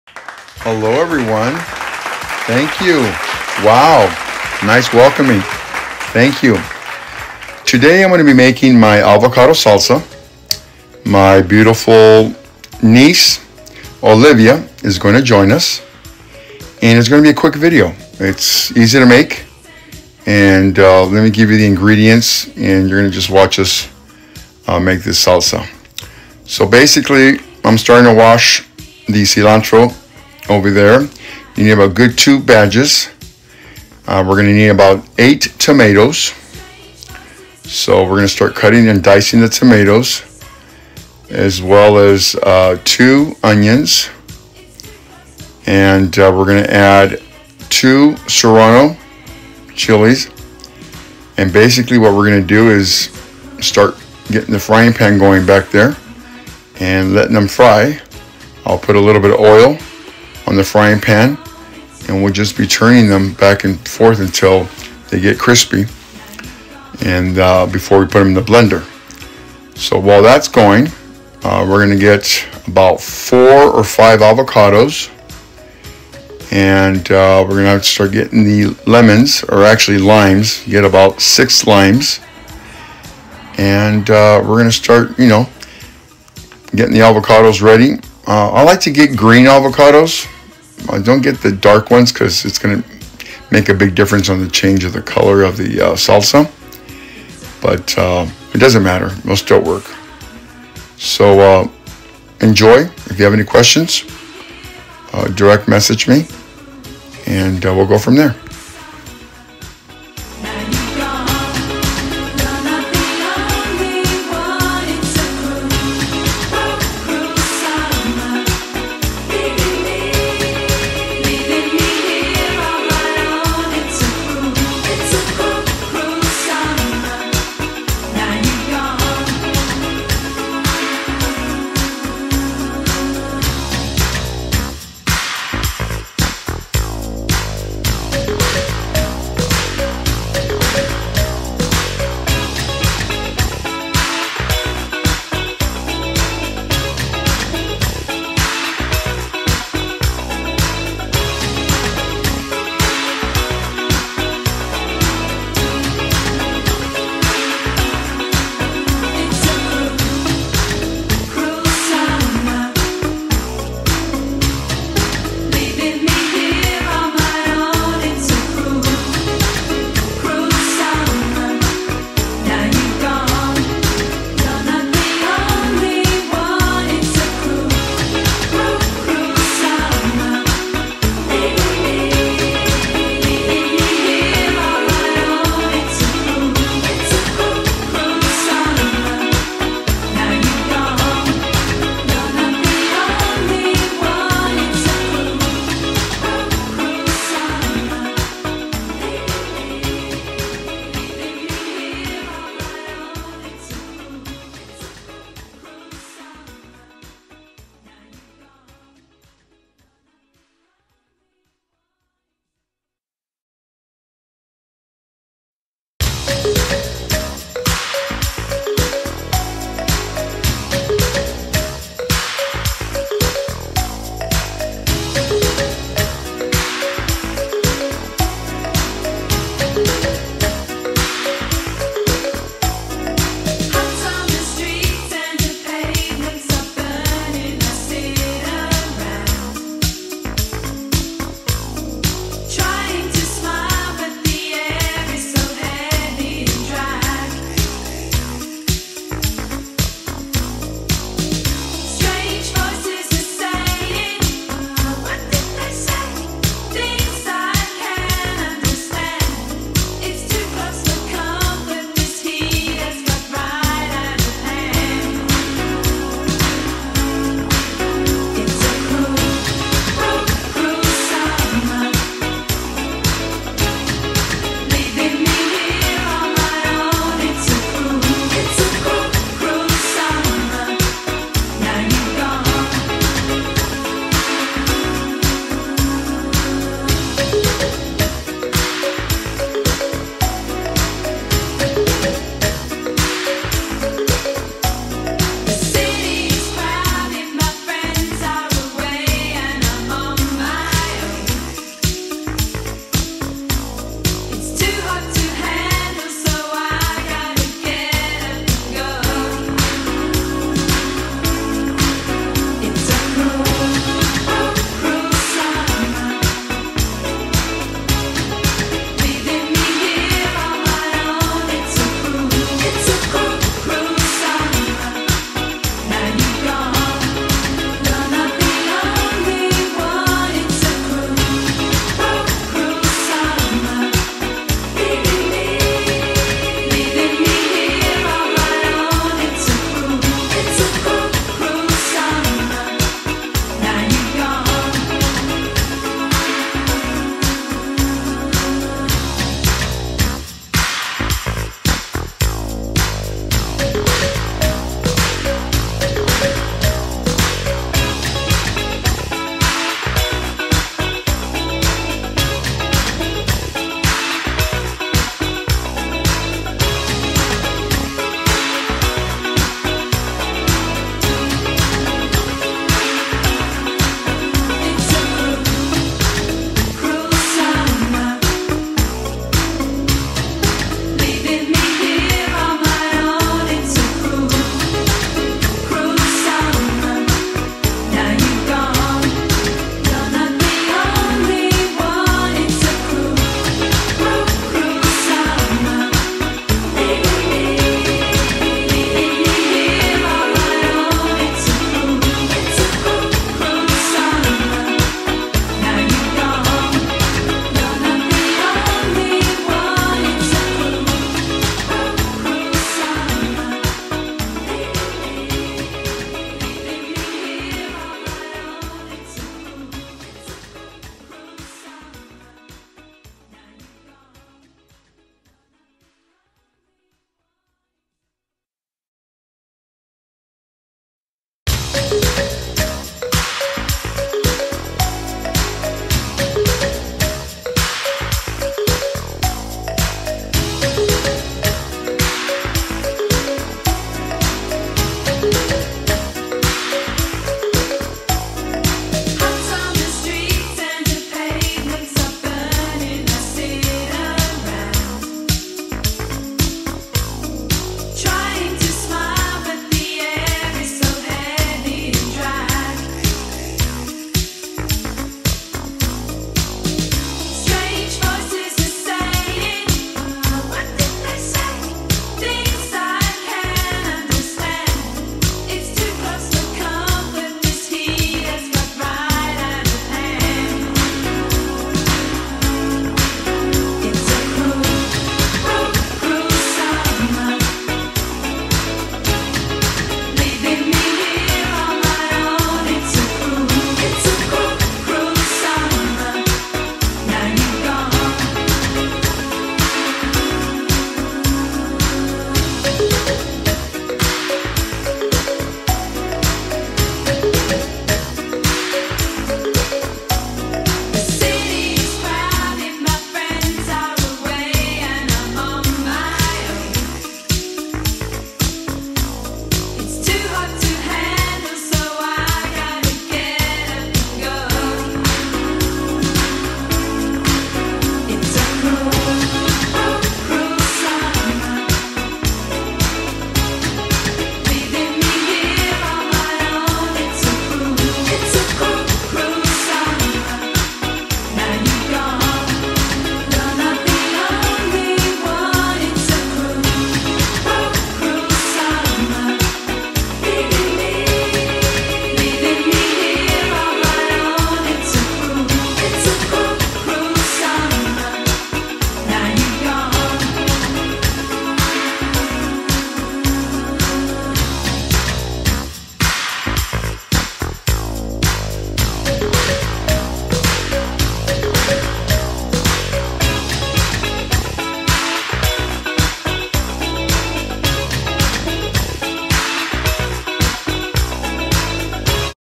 ZEKE- Avocado 🥑 Salsa
ZEKE - making his famous Avocado 🥑 Salsa with his God Daughter Olivia.